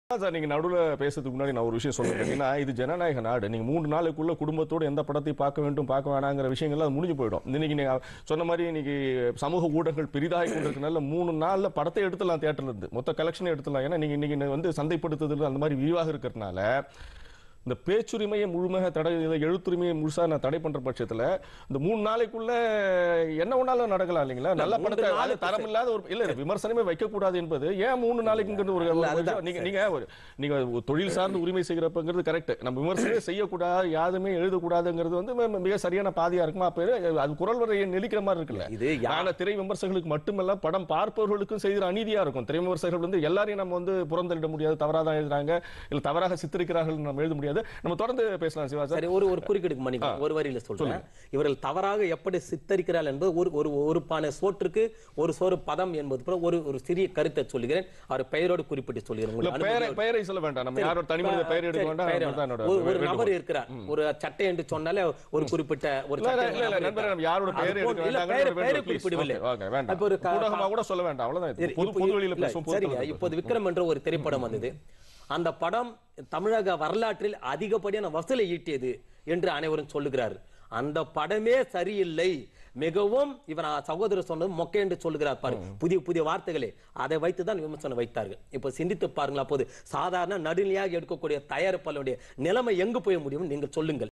தவறாக விமர்சிப்பதா ? - ஜி.ஜி.சிவா கோபம்
திரை விமர்சனத்துக்கு கட்டுப்பாடா ? - தயாரிப்பாளர்கள் , திரை விமர்சகர்கள் பங்கேற்ற காரசார மக்கள் மேடை விவாதம்